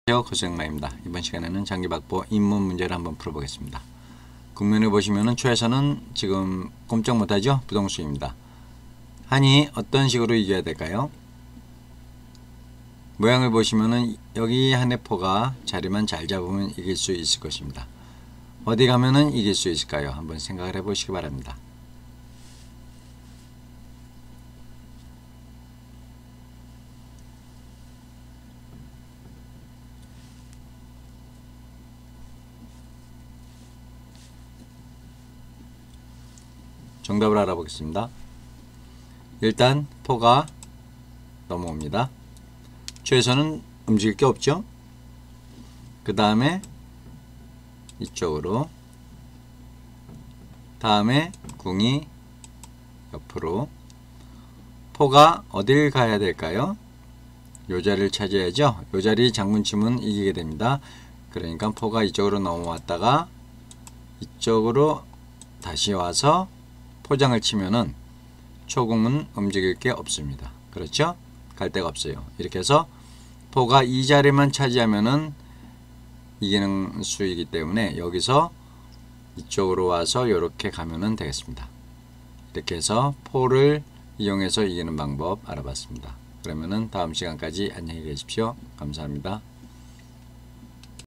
장기[입문] - 부동수문제 포위치찾기 [장기초보]
상대는 부동수 입니다. 포의위치만 잘잡으면
간단히 이길수 있습니다 ~~